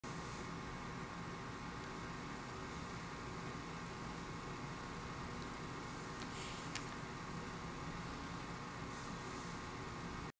Big yawn: 9 days after broken lower jaw wired together
This is Mr. Foster yawning with a broken lower jaw. It was wired together 9 days ago